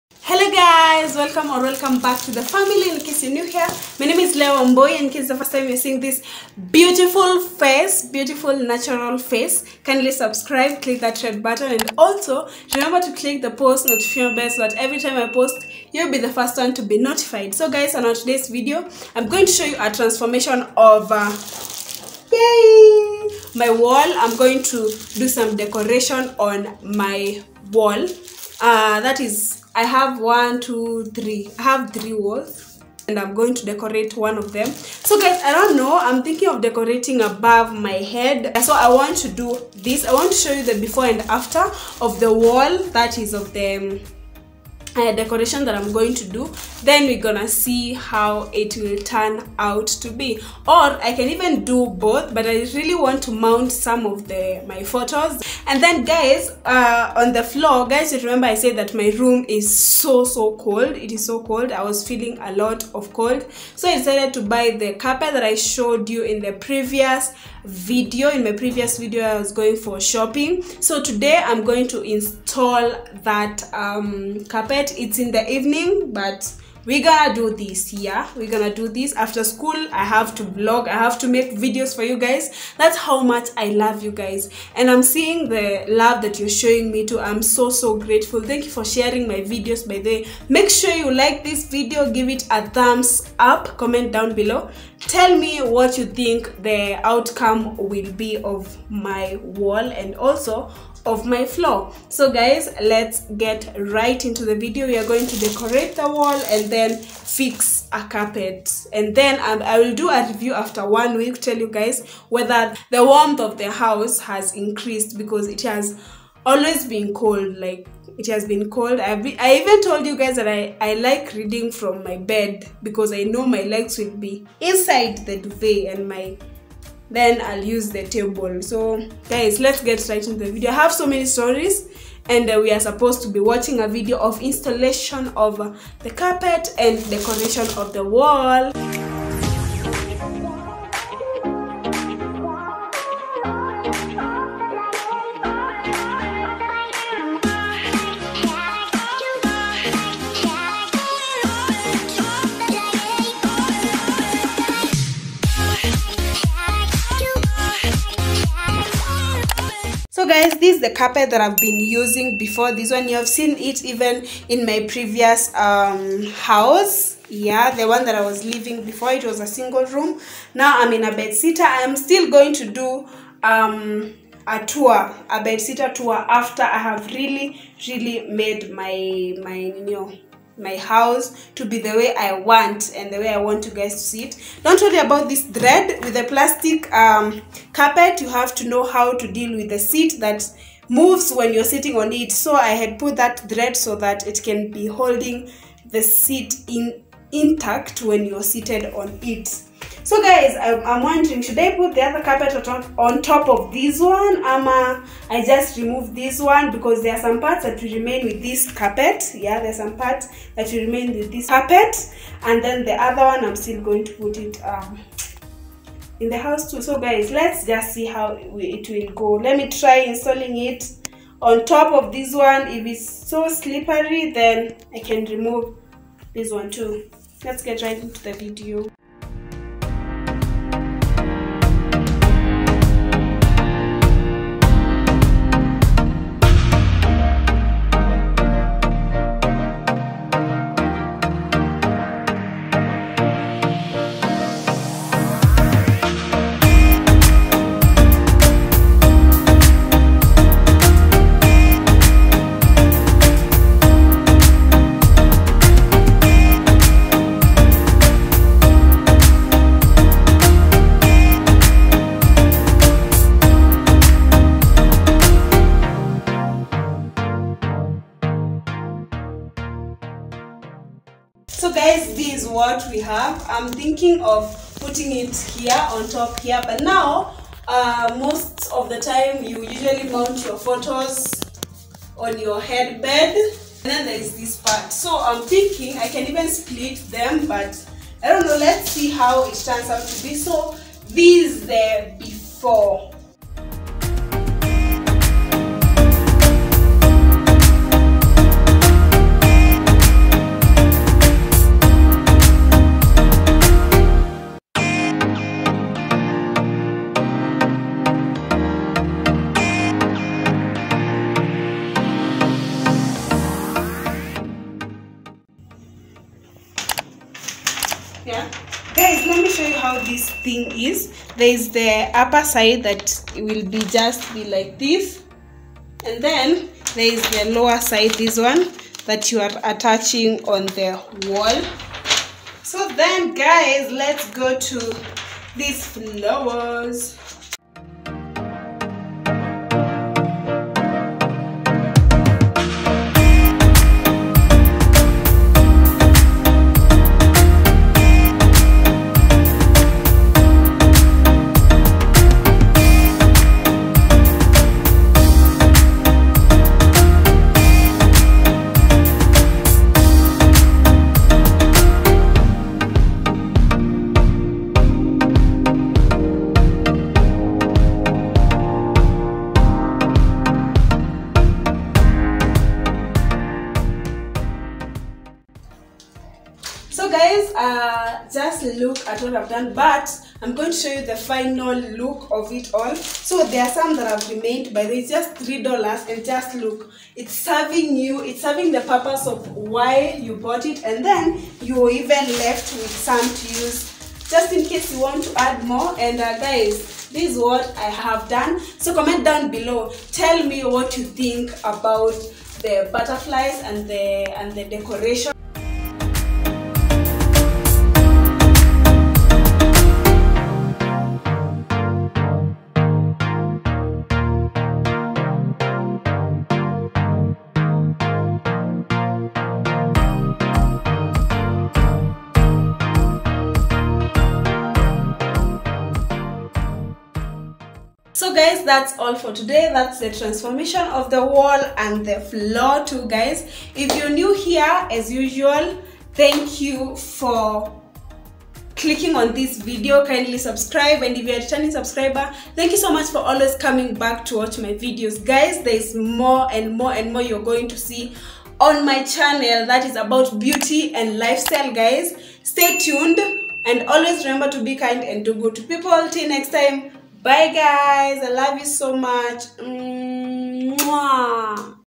Floor TRANSFORMATION and Wall DECORATIONS in a Bedsitter Apartment
Enjoy the video, watch till the end

If you love the video and the work I put into it, please consider subscribing, liking the video and let me know in the comment section what you think about the video.

The lighting in this video is done using a ring light.